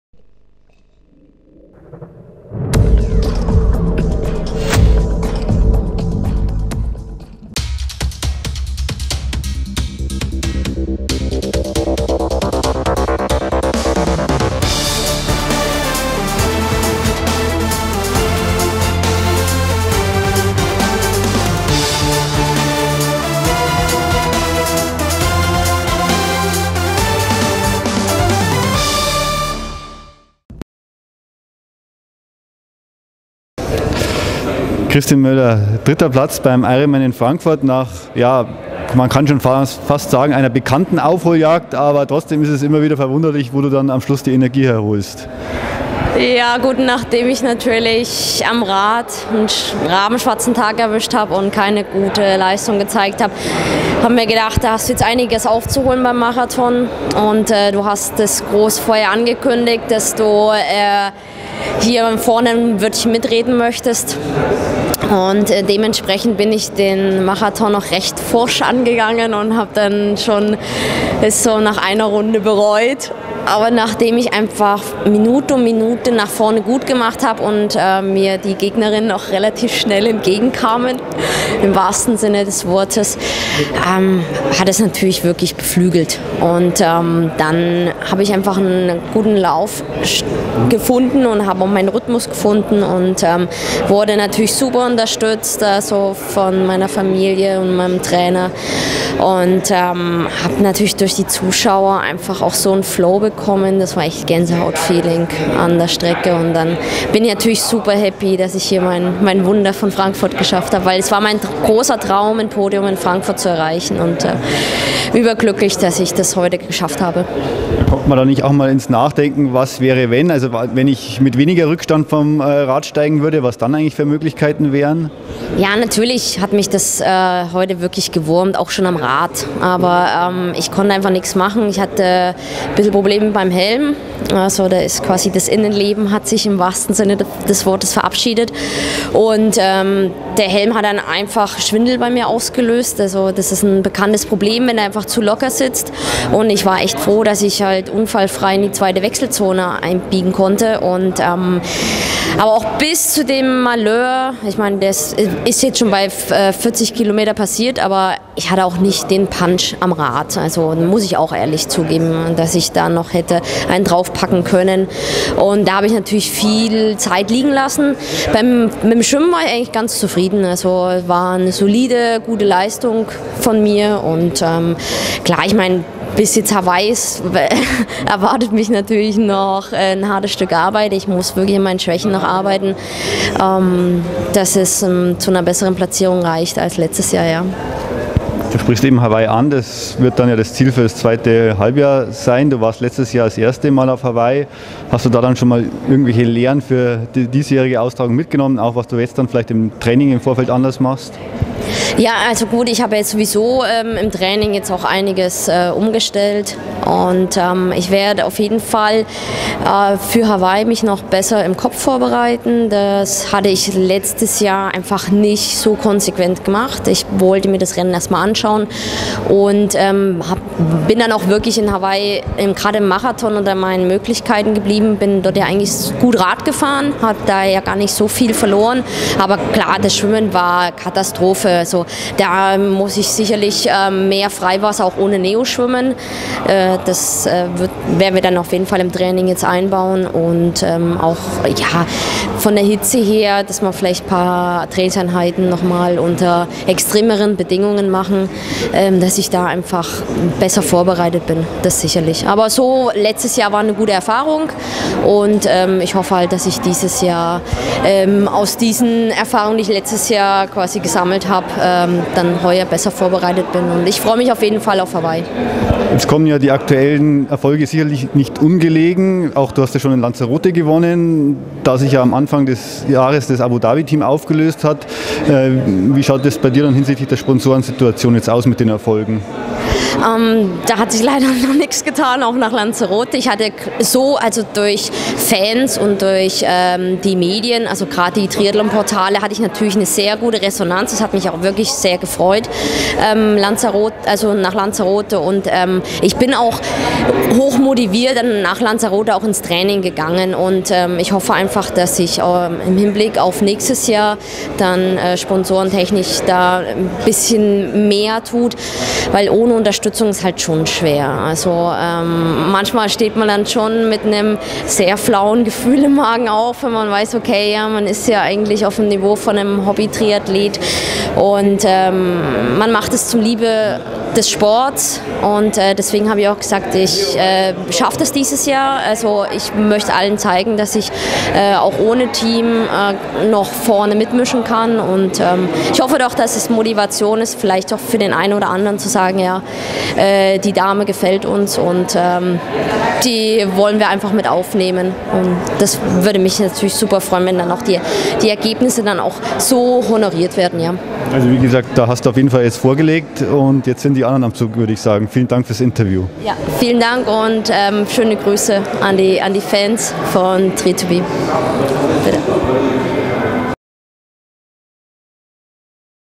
Ironman Frankfurt 2013: Interview Kristin Möller (3. Platz)
Kristin Möllers lief mit einem 2:57 Stunden Marathon noch auf Rang drei beim Ironman Frankfurt 2013 ...